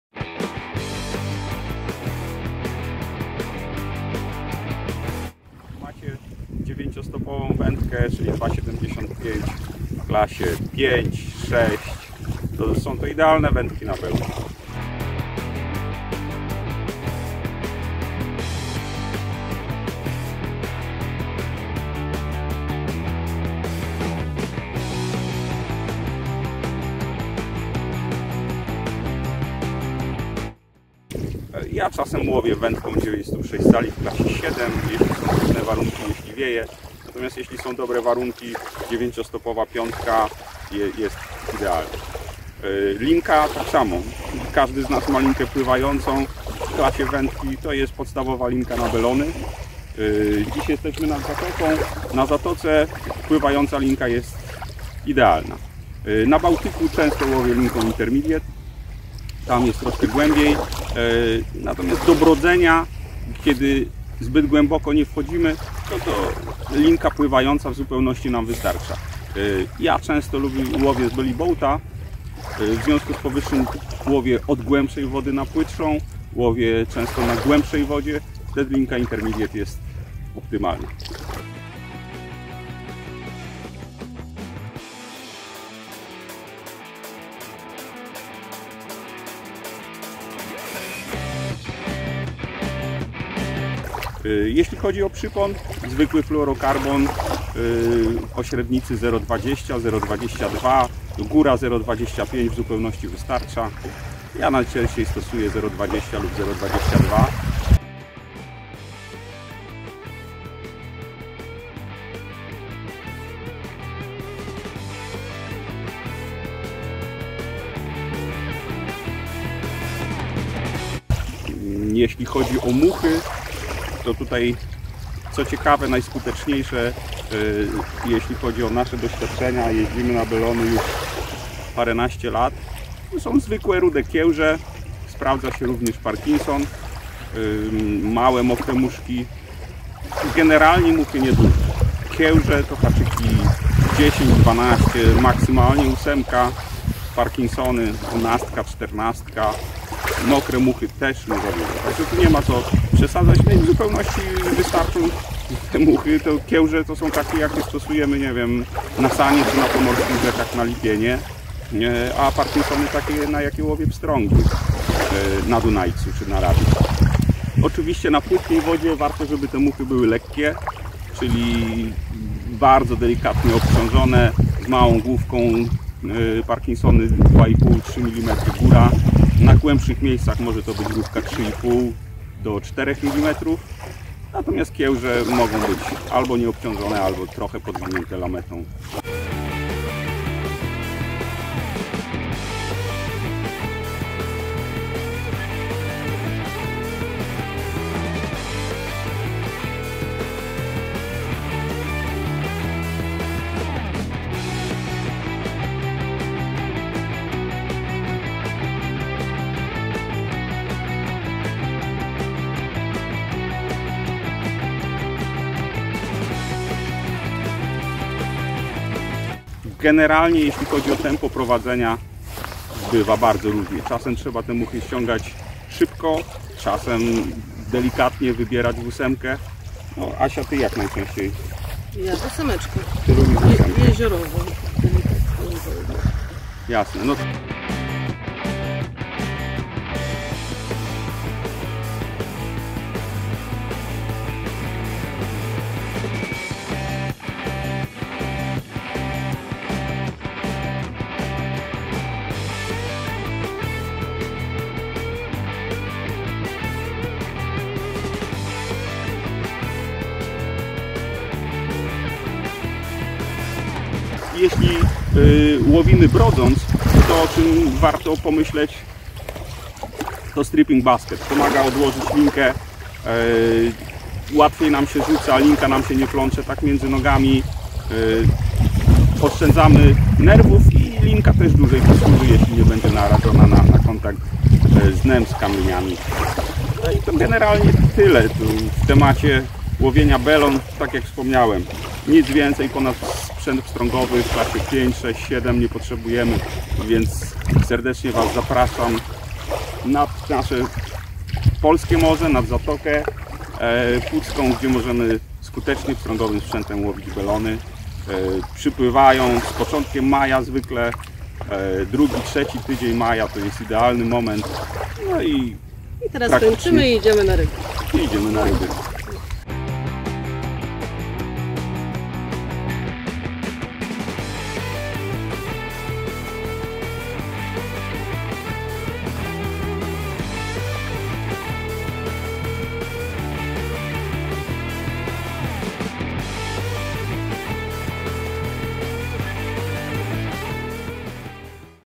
👍Belony na muchę w Bałtyku - Fly fishing for garfish!
Jak złowić belonę na muchę? W powyższym filmie opowiadam o moich kilkunastoletnich doświadczeniach z łowienia belon na sztuczną muchę w Bałtyku. Dowiecie się jaką wędkę wziąć ze sobą, jakiej użyć linki i żyłki na przypony, jak prowadzić muchę, gdzie i kiedy szukać belon. A na jakie muchy biorą najlepiej? Tego też dowiecie się z filmu. Zapraszamy!

How to catch garfish on a fly? In the above video, I talk about my many years of fishing for garfish with a fly in the Baltic Sea.
You will learn what rod to take with you, what line or leader to use, how to strip a fly, where and when to look for garfish.
And what flies do they catch best? You will also learn this from the movie. We invite you!